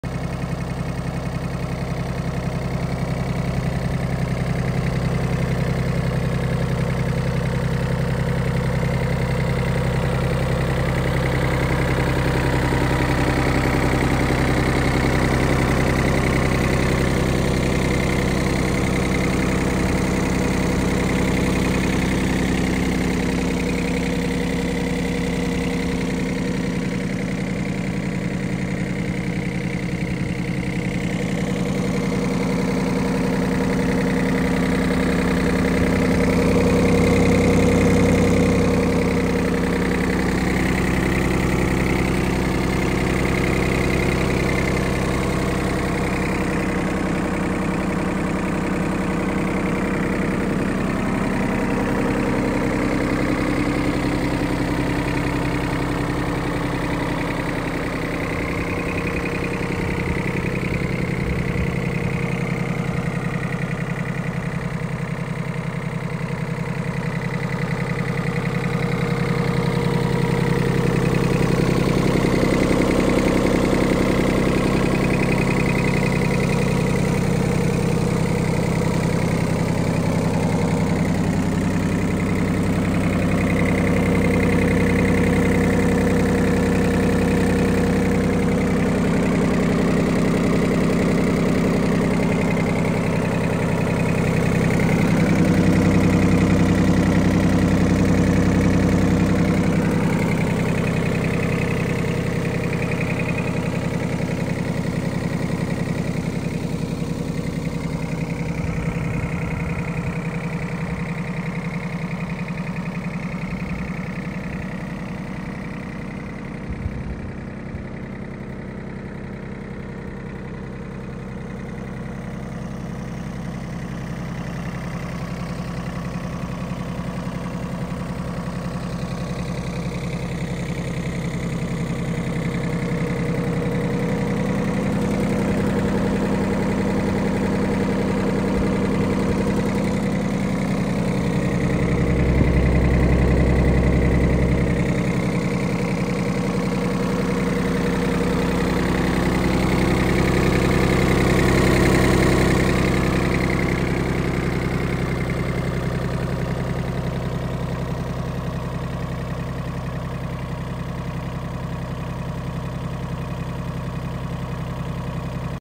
YANMAR AF17 4x4 Compact Tractor & New 4ft Flail Mower
Very Nice 4x4 Yanmar 3 cylinder diesel tractor, super clean.